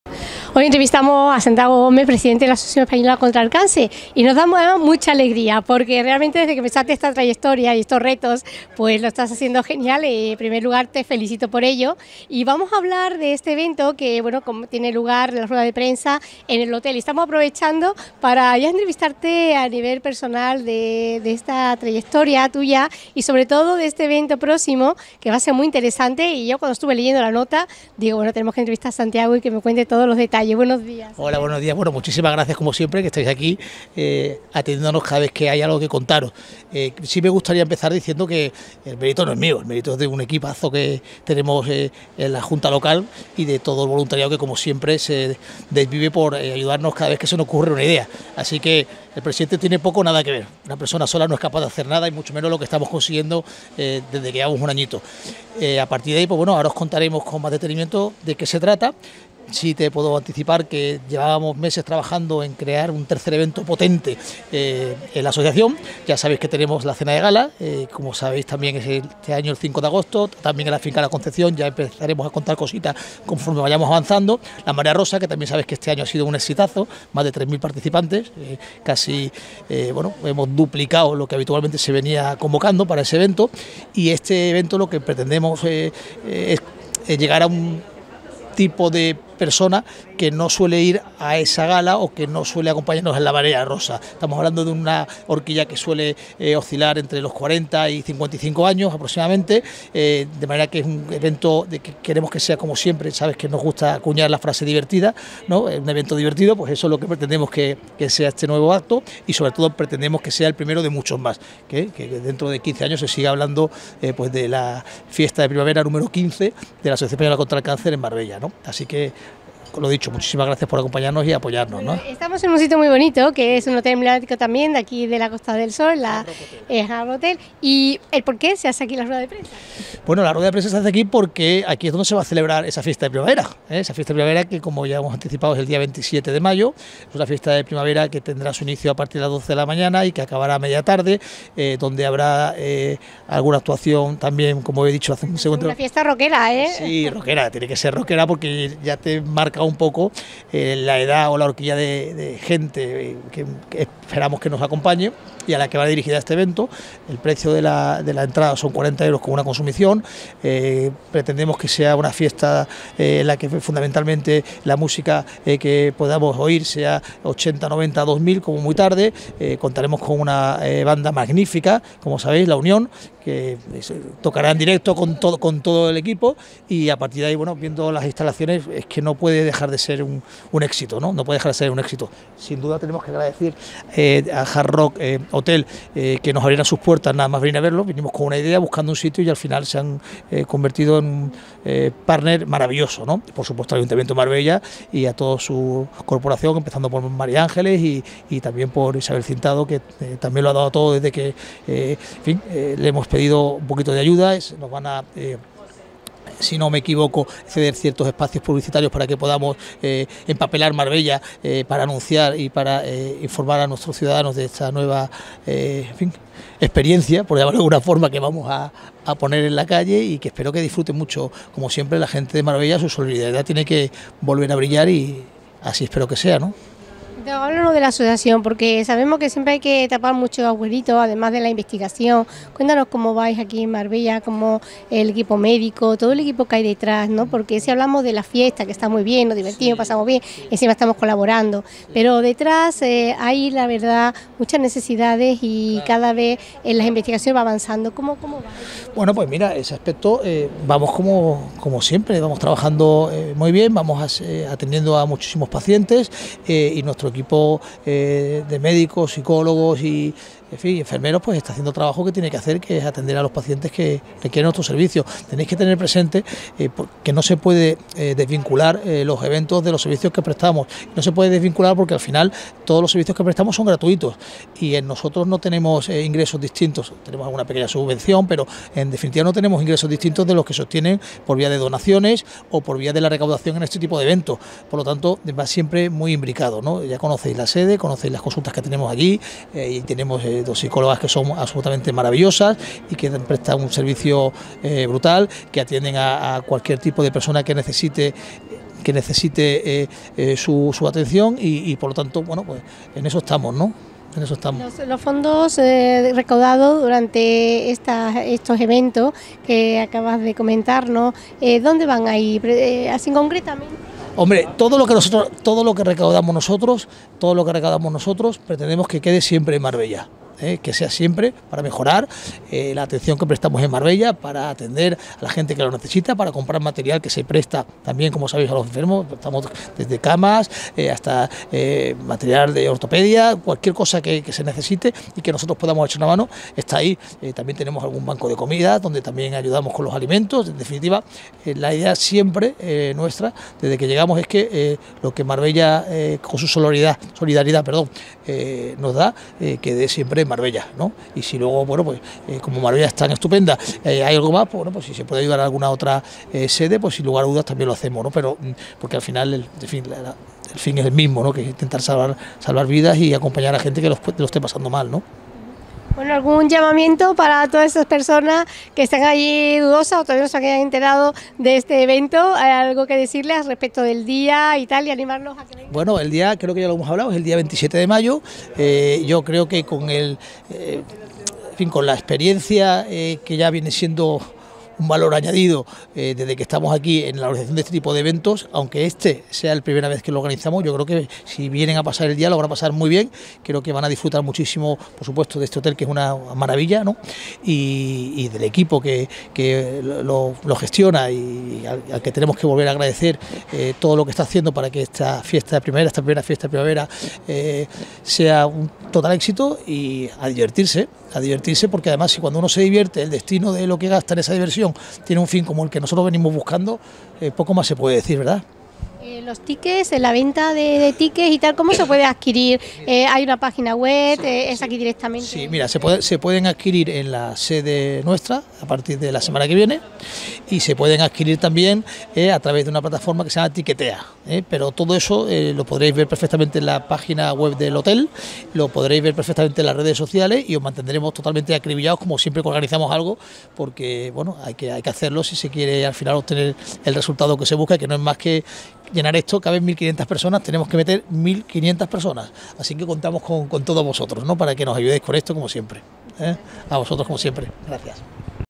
Entrevista de Helena Olaya a Santiago Gómez Villares Presidente de la AECC Marbella-fiesta de la pri
El evento solidario tendrá lugar el 27 de mayo, desde las 14:00h hasta el atardecer, en la piscina del hotel y será el primero de la temporada de conciertos Hard Rockollection. Todos los beneficios irán destinados a los programas y servicios gratuitos que la asociación presta en Marbella.

La fiesta contará con música de los 80-90 a cargo del DJ Kike Supermix y un concierto en directo del grupo La Unión.

El objetivo es consolidarla como el tercer gran evento de la Asociación en Marbella junto a la Cena de Gala de agosto y la carrera Marea Rosa de noviembre.
El Ayuntamiento de Marbella, colaborador habitual en las actividades de la Asociación, ha estado representado por la alcaldesa Ángeles Muñoz. En esta ocasión ha cedido soportes publicitarios, estáticos y digitales, para la difusión del evento.

Las entradas ya están a la venta en la sede de la asociación en Marbella, en la web de Hard Rock Hotel Marbella y en la plataforma entradas a tu alcance, al precio de 40€ con una consumición incluida. Habrá numerosas sorpresas y una rifa donde todos los participantes tendrán la oportunidad de ganar magníficos premios.